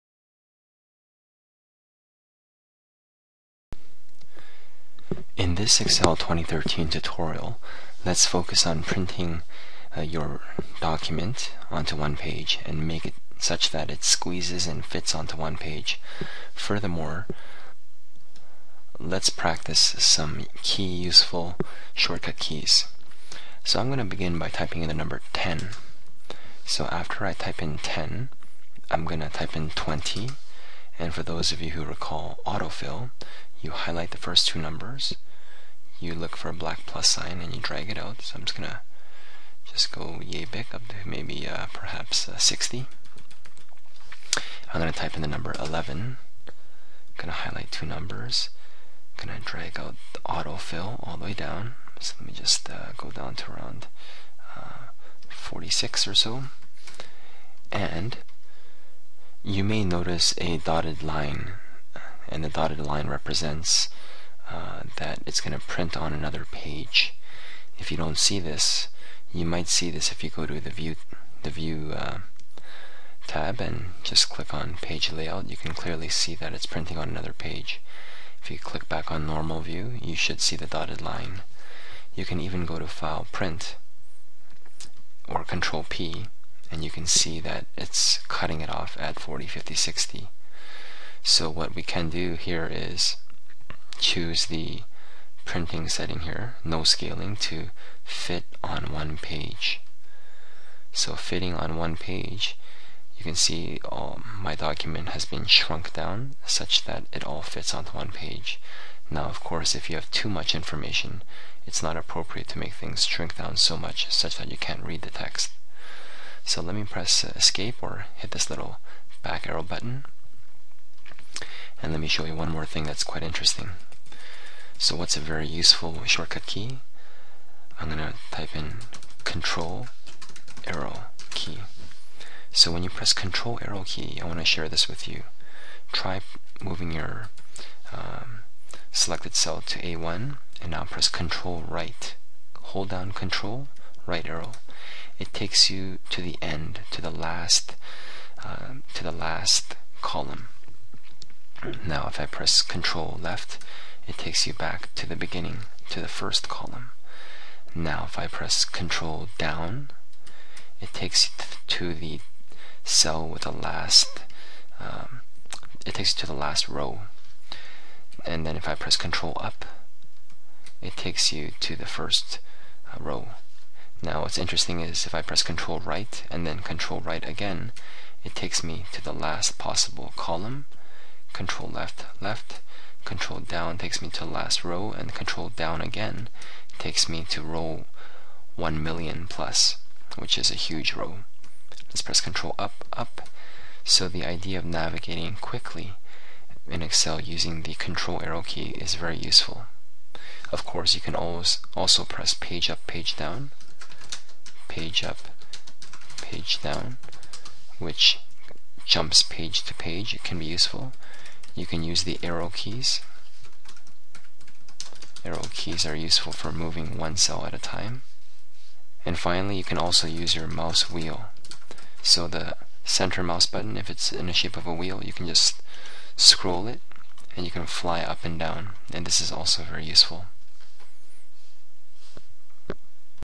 Excel 2013 Tutorial - Force Printing on One Page and Navigating the Worksheet
Excel 2013 Tutorial for Beginners - Force Printing on One Page and Navigating the Worksheet.